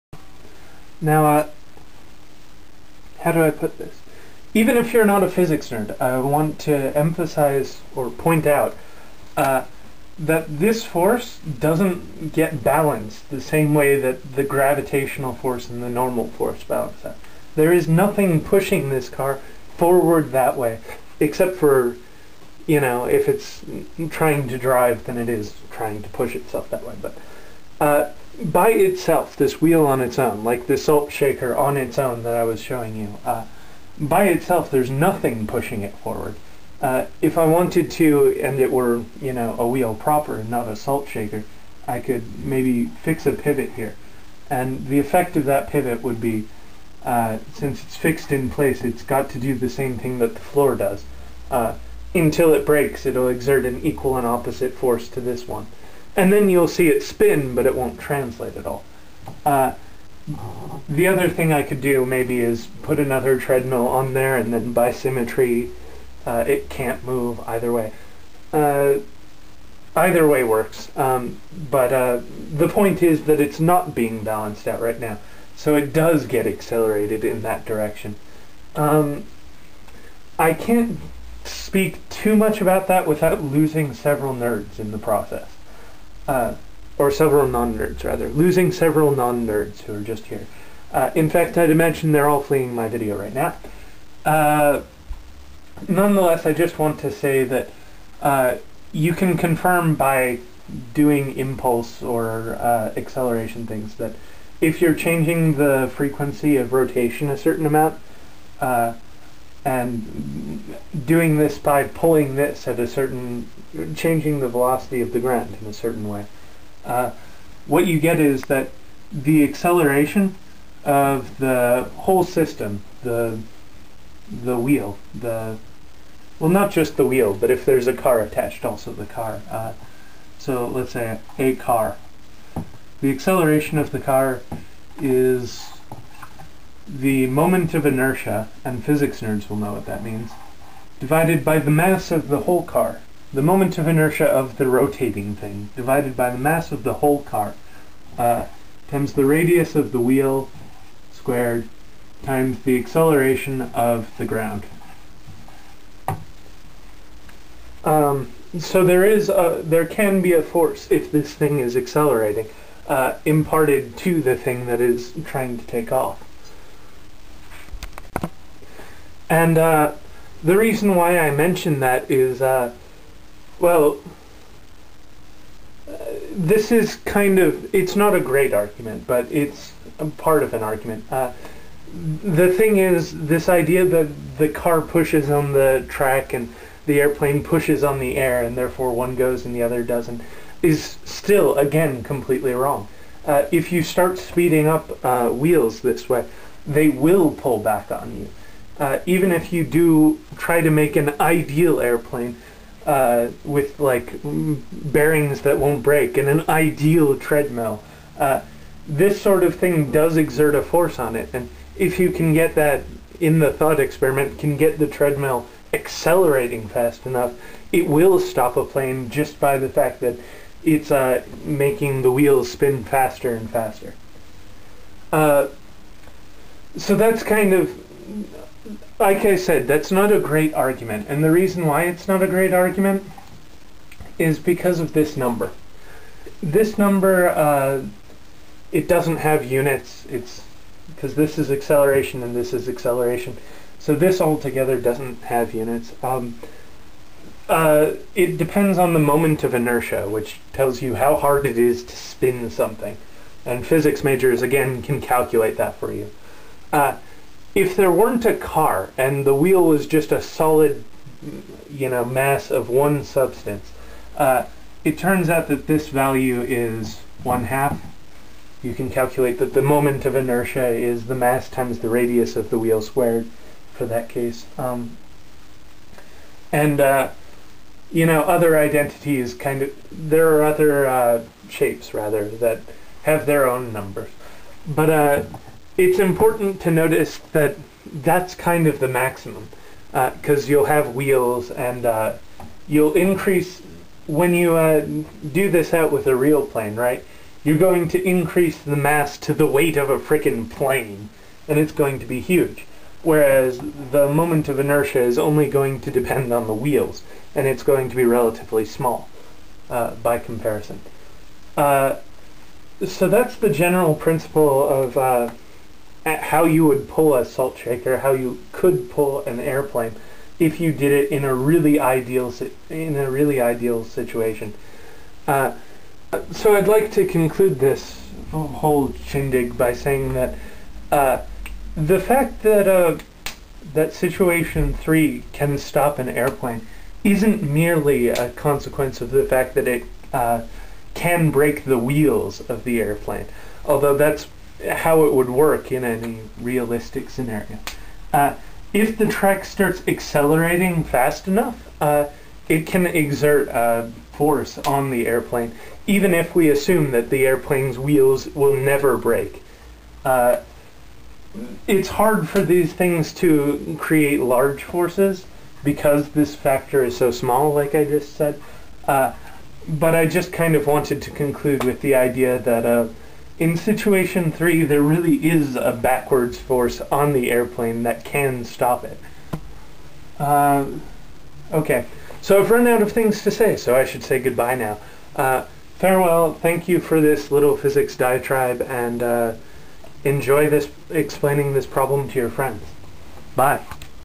Airplane on a Conveyor Belt - Complete Physics, pt. 4
The final part in a complete description of the Airplane on a Conveyor Belt Problem